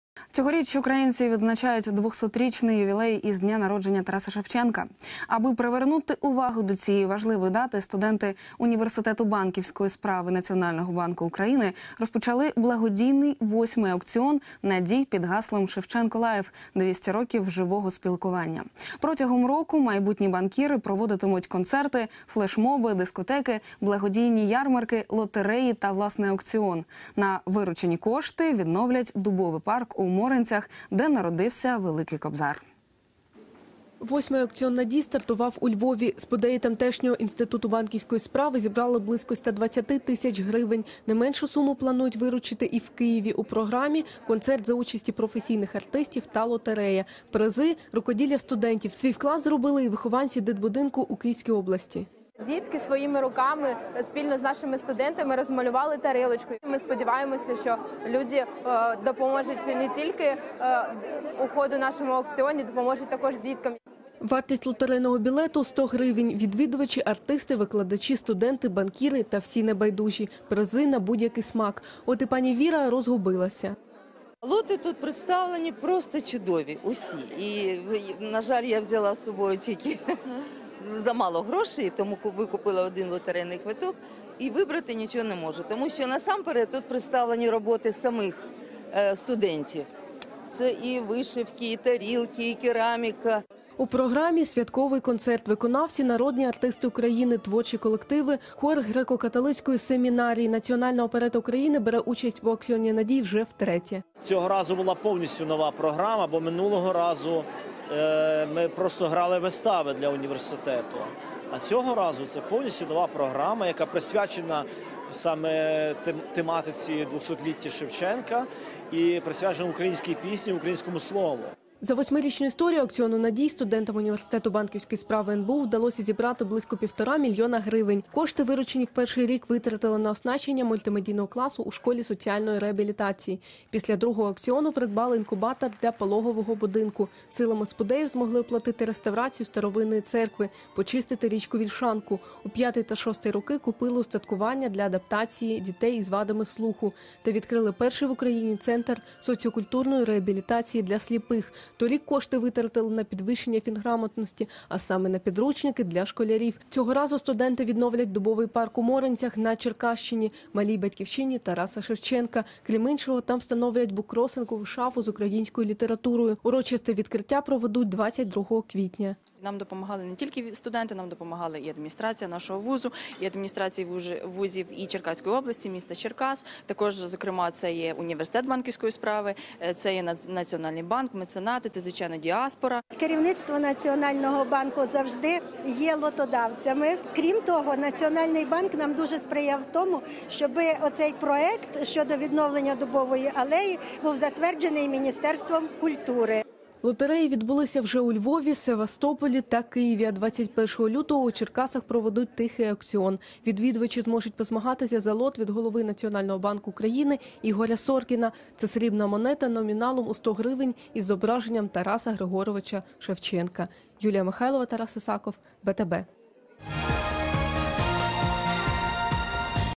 «Шевченко -- Live. 200 років живого спілкування» Телеканал "БТБ"
Благодійний проект «Шевченко -- Live. 200 років живого спілкування» на сцені Національної оперети України 07.02.14